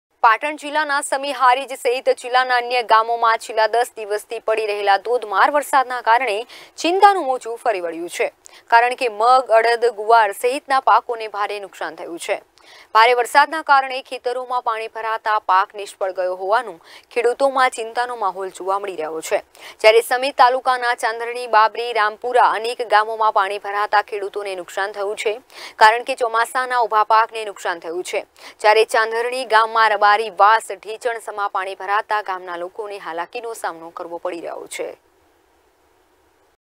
News | પાટણ જિલ્લામાં ભારે વરસાદથી ખેતરોમાં પાણી | VR LIVE
Reporting by  Sachin Pandya

Voiceover - Mittal  |  Copy Editor - Gopal

Edited By - Vipul  |  Uploaded By - Hetu